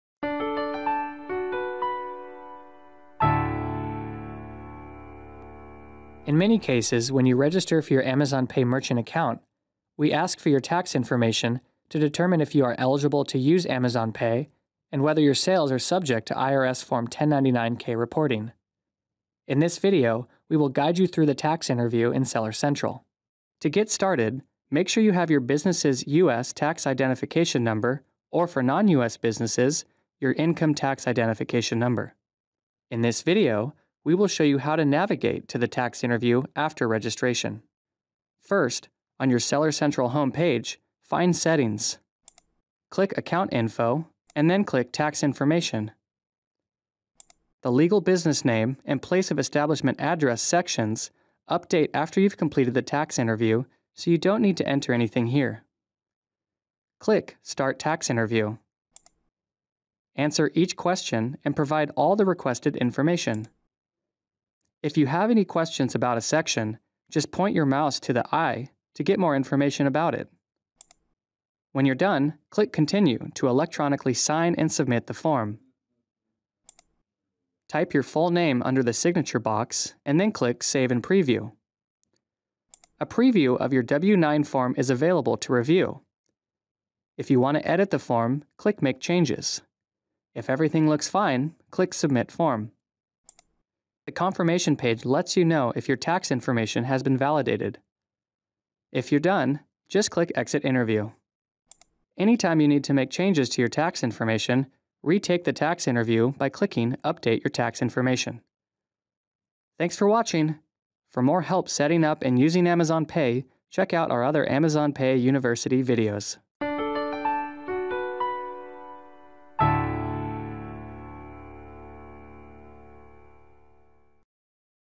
Completing the tax interview in Seller Central
In this video, we will guide you through the tax interview in Seller Central.

Support:
If you have any questions please reach out to our support team